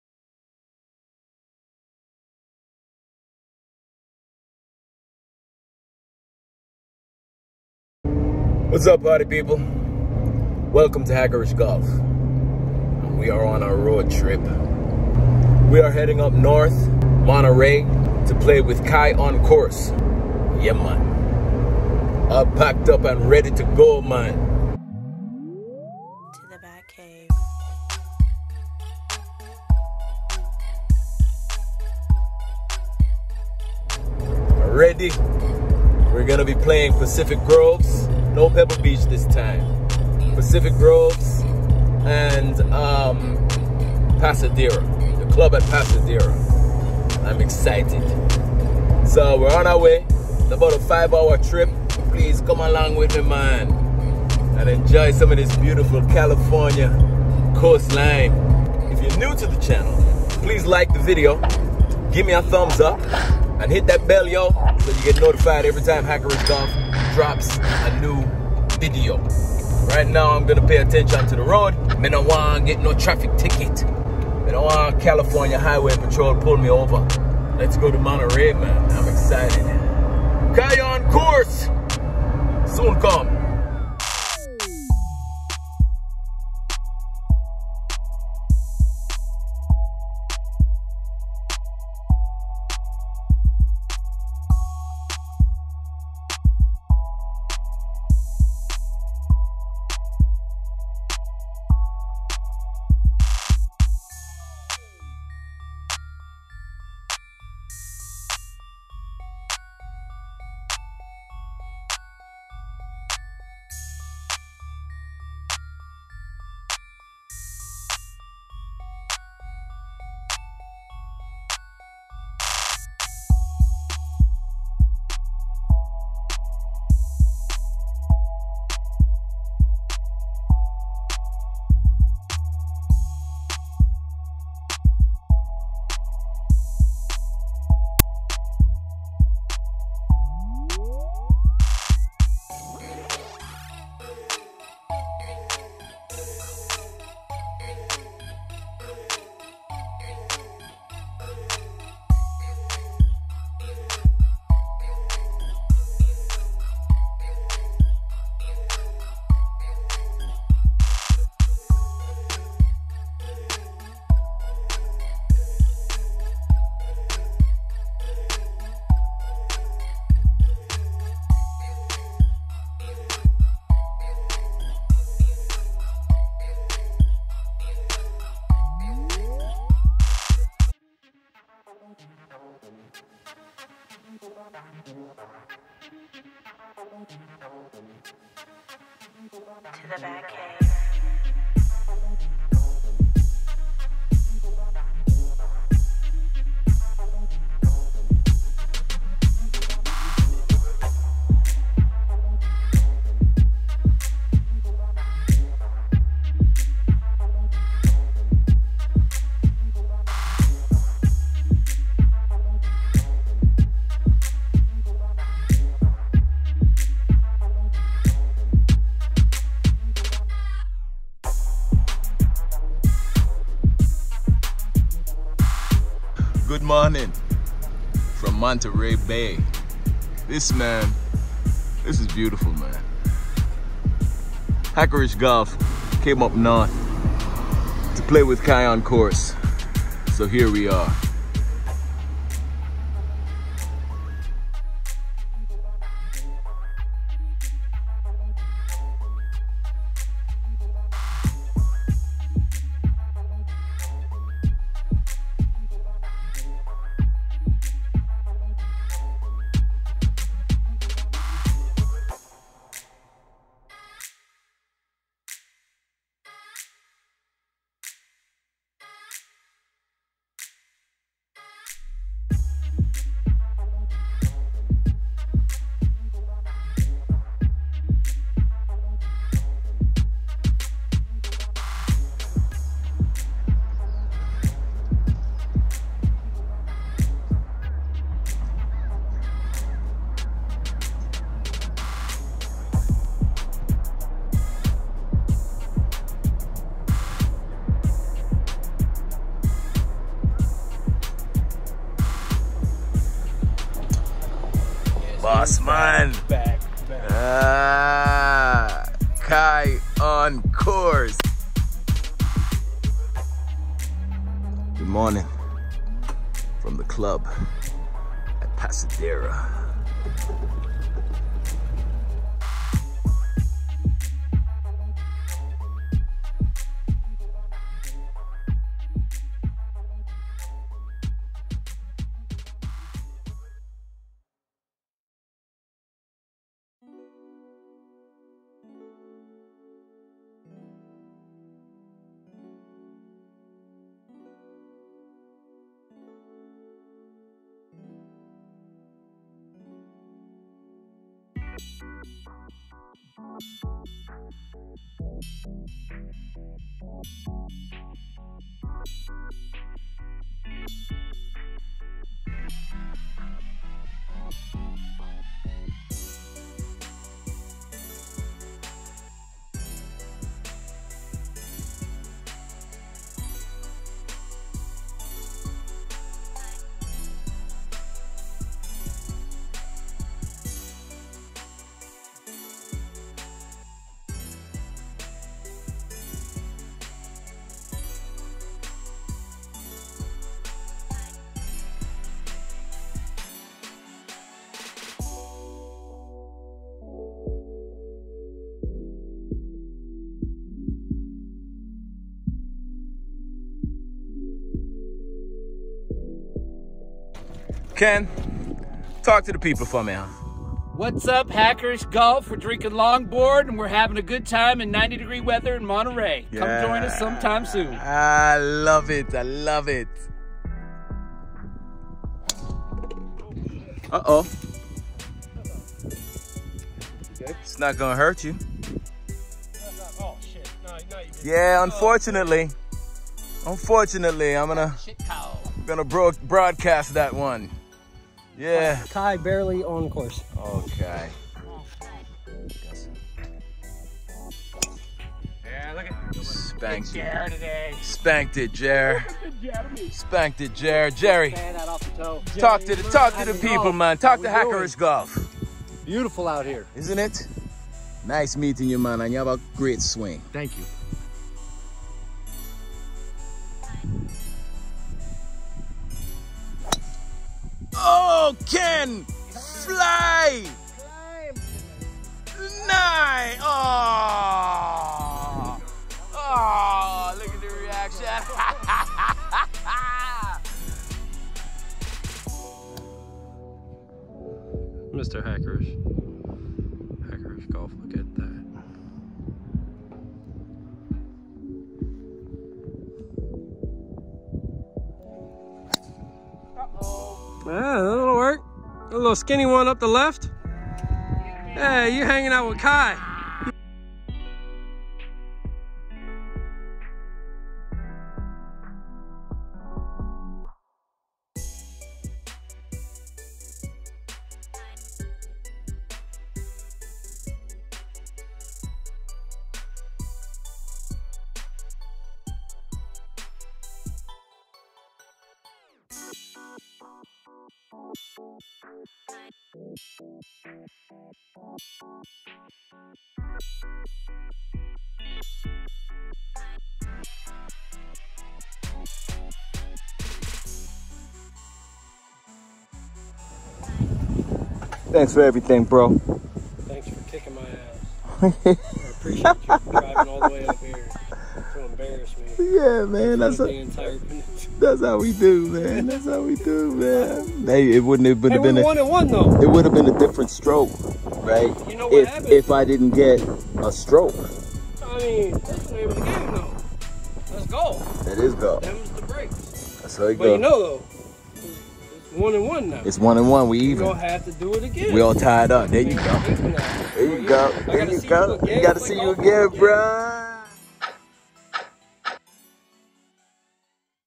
Monterey Take Over Part 2
Hackerish Golf goes to Monterey to play a couple of rounds with Kyoncourse at The Club at Pasadera and Pacific Grove Links.